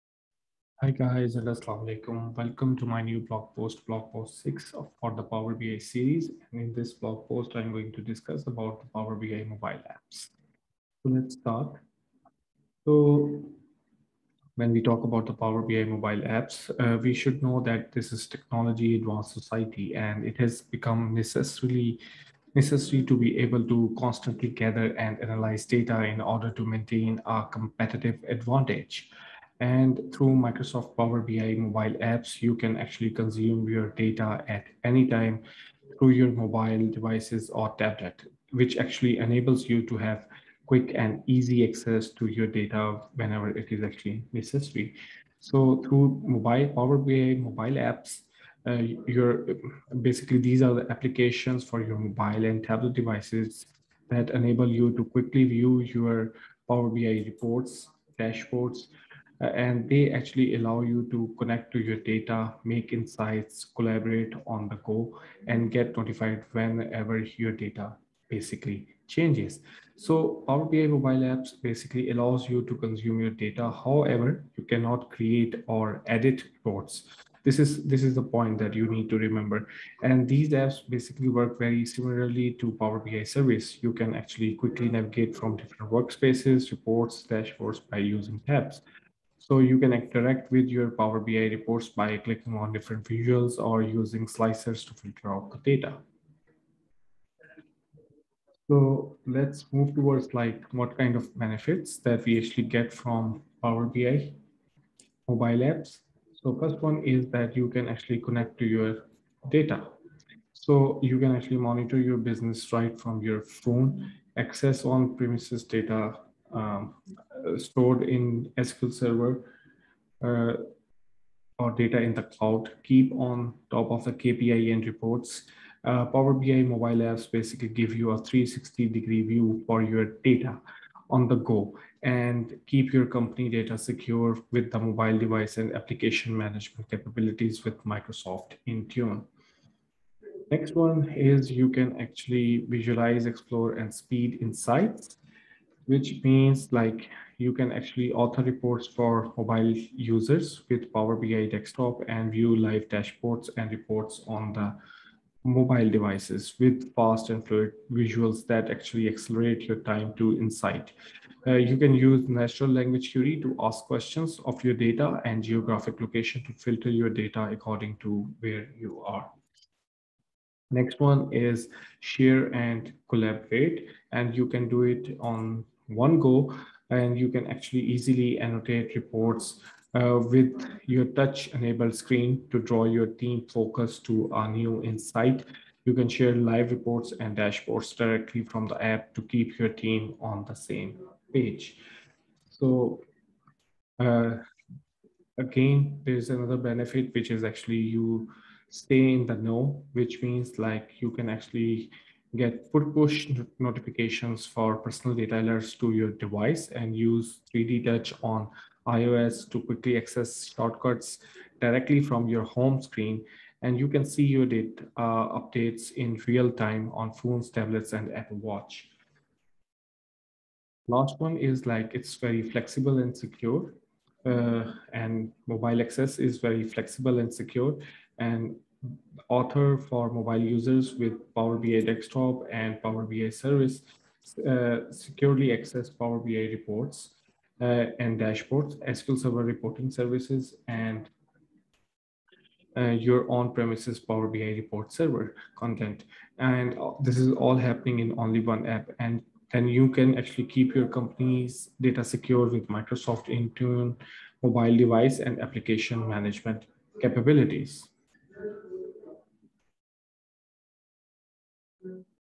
Power BI Mobile Apps | Power BI Mobile Apps Tutorial
Power BI Mobile Apps

 . Quickly View
 . Reports
 . Dashboards
 . Data
 . Make Insights
 . On the Go
 . Notified

Connect to your data
 . Monitor your business right from your phone

Visualize, explore, and speed insights
 . Author Reports

Share and collaborate
 . Annotate Reports

In the Know
 . Push notification

Flexible and Secure
 . Secure access Power BI Reports
 . Dashboards
 . SQL Sever Reporting Services

_________________ Latest MSSQL Videos ________________